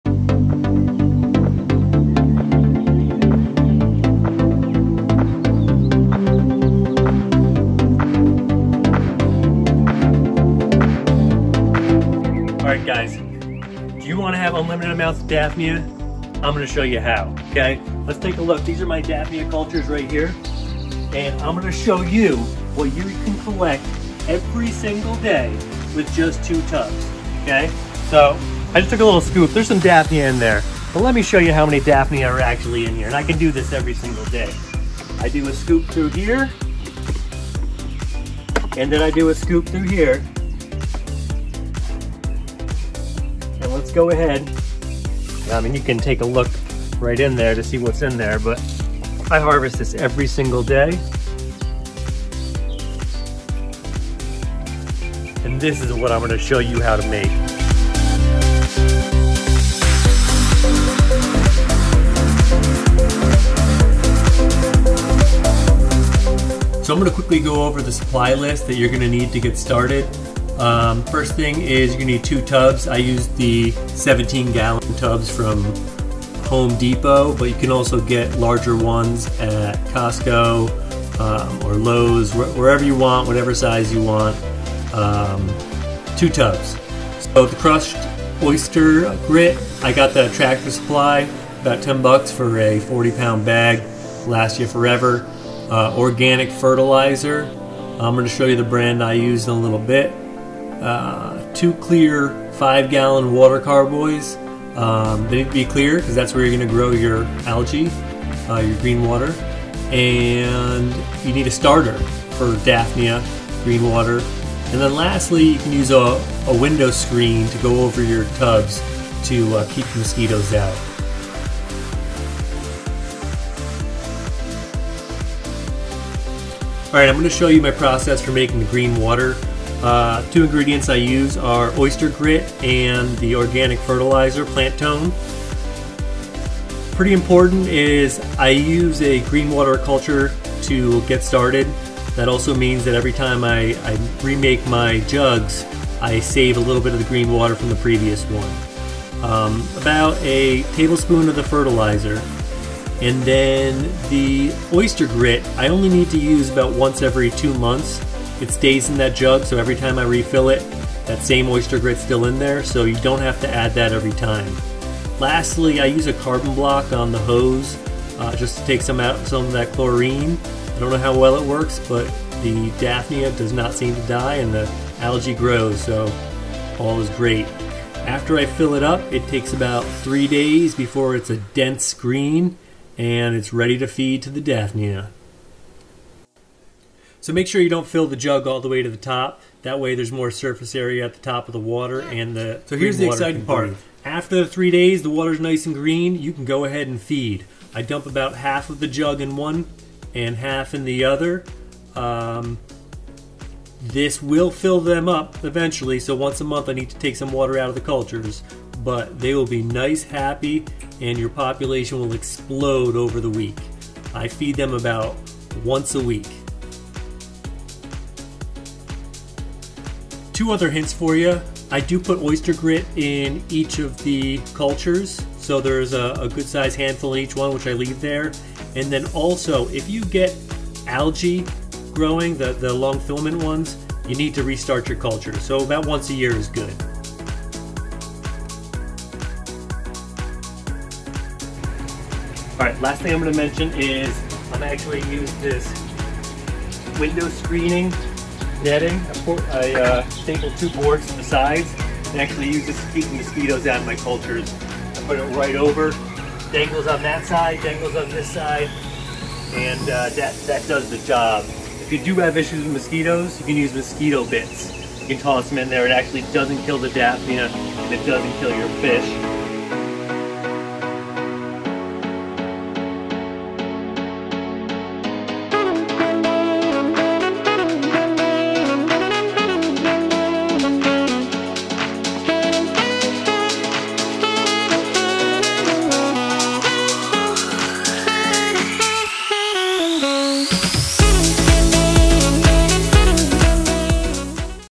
Daphnia Explosion - How to Raise Daphnia and Feed Your Entire Fish Room
I document my most current method. I'm done with indoor raising, and done with spirulina, yeast and rice flour. This method WORKS!